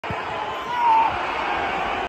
B.93 TV: Sebastian Clemmensen kommer fri, men får ikke træf på afslutningen
Stor chance til Sebastian Clemmensen, som dig rammer afslutningen helt skævt med venstreskøjten. Sebastian tog dog revanche med 1-0 scoringen i 2. halvleg, som blev køligt sat ind på et lob efter en sukkerbold fra Jeppe Erenbjerg i dybden. B.93 vandt 2-0 hjemme over FC Helsingør fredag 10. november 2023.

🎥 Rasmus Bendtsen.